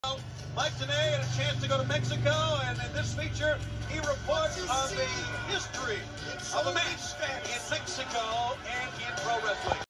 Eddie Guerrero Looks into Corner Camera & Says What you see is only Fantasy after being victorious
TGOTGOGS 1 WCW 97